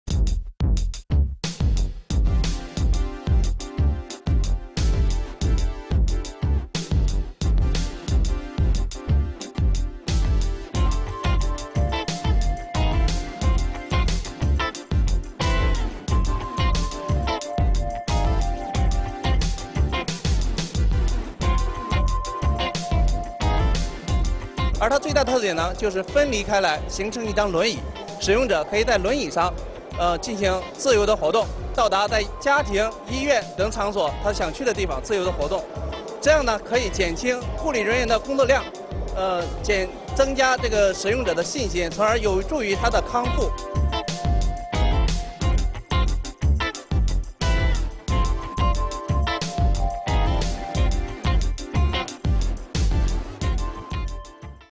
ဝွီးခ်ဲ ႏွင့္ အိပ္ရာလိုသလိုေျပာင္းလဲေပးေသာ နည္းပညာသံုးစက္ပစၥည္း
ေပက်င္း၊ ၾသဂုတ္ ၂၉ ရက္ (ဆင္ဟြာ)
ခလုတ္တစ္ခုႏွိပ္ရံုျဖင့္ အိပ္ရာမွ ဝွီးခ်ဲ ကုလားထိုင္သို႔ အလိုအေလ်ာက္ ေျပာင္းလဲႏိုင္ေသာ နည္းပညာသံုးစက္ပစၥည္းကို ေပက်င္းတြင္က်င္းပလ်က္ရွိေသာ ၂ဝ၁၇ ခုႏွစ္ စက္ရုပ္မ်ားညီလာခံတြင္ျပသခဲ့သည္။ ရွန္ယန္းအေျခစိုက္ SIASUN Robot and automation ကုမၸဏီက အဆိုပါ နည္းပညာကိုတီထြင္ဖန္တီးထားျခင္းျဖစ္သည္။ သြားလာလွဳပ္ရွားရန္ခက္ခဲသူမ်ား မသန္စြမ္းသူမ်ား၊သက္ႀကီးရြယ္အိုမ်ား ကို ပိုမုိအဆင္ေျပသည့္ကူညီမွဳမ်ားေပးရန္ဒီဇိုင္းထုတ္ထားျခင္းျဖစ္သည္။ (Xinhua)